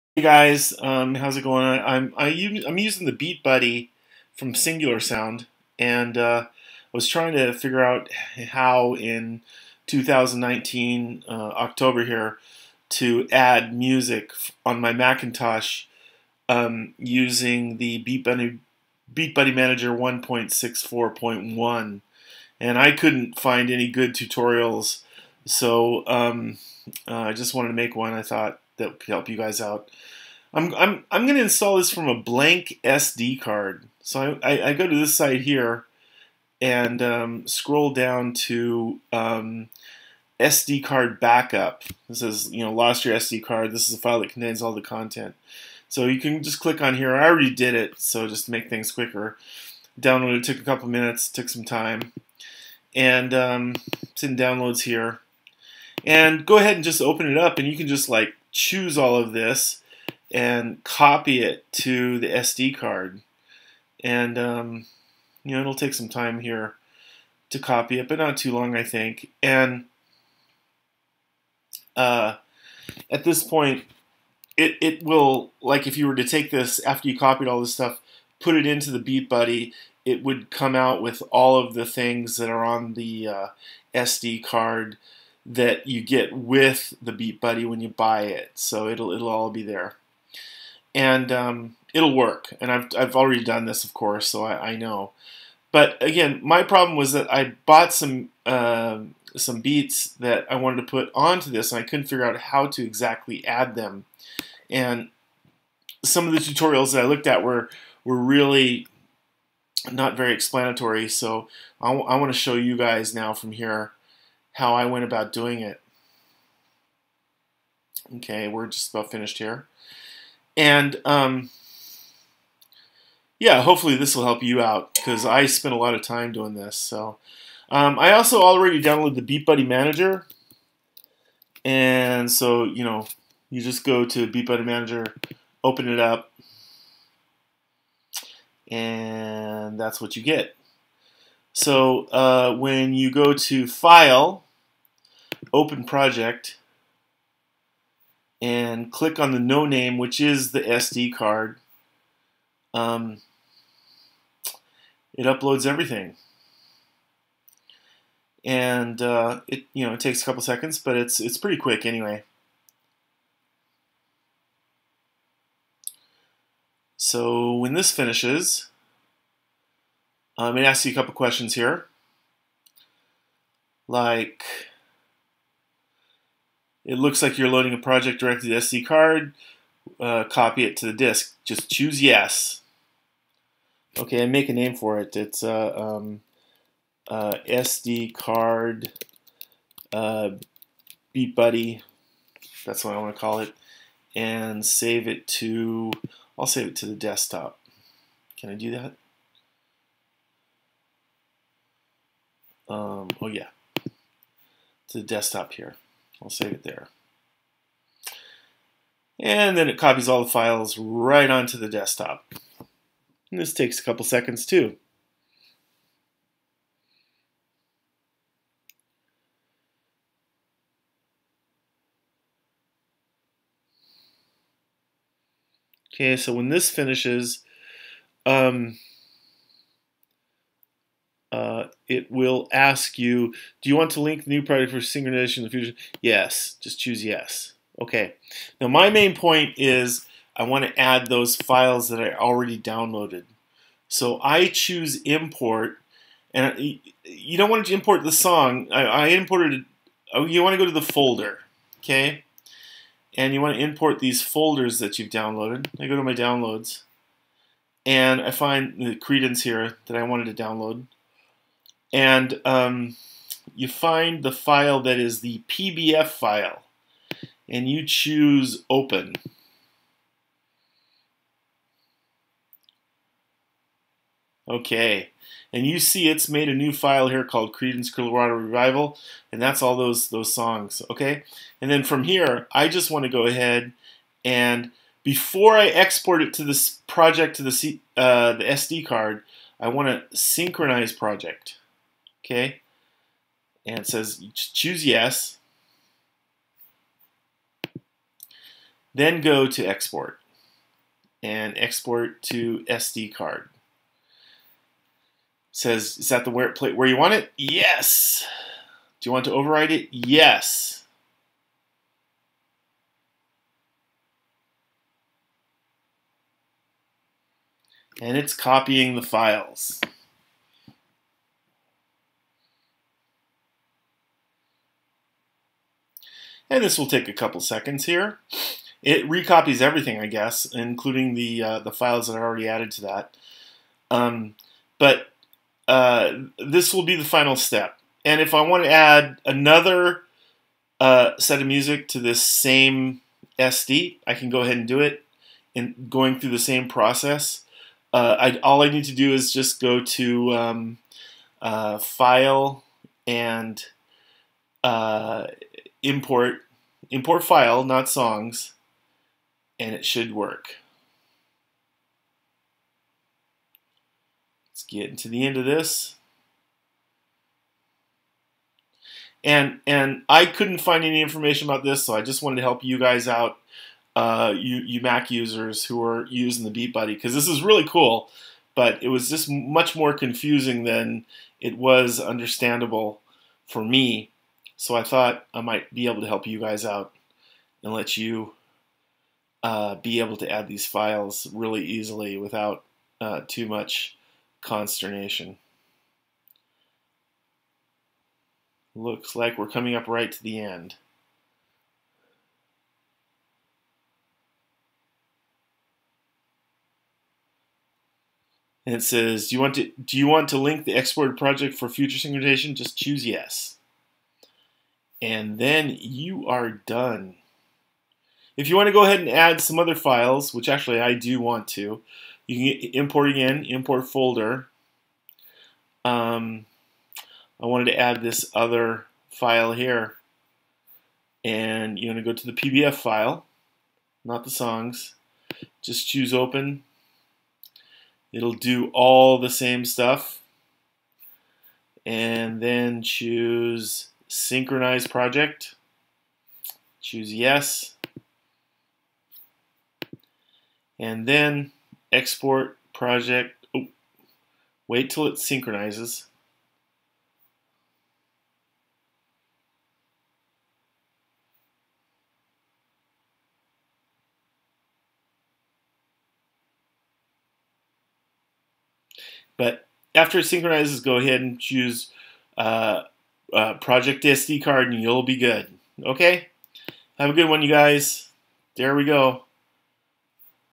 Beatbuddy Manager 1.64 Mac software. Using your Macintosh to import purchased songs
I use a Beat Buddy and a mac.  Here it is October 2019.  I could not find any good tutorial online.  I decided to make my own.  Watch this to learn how to import or upload songs to your Beatbuddy drum pedal.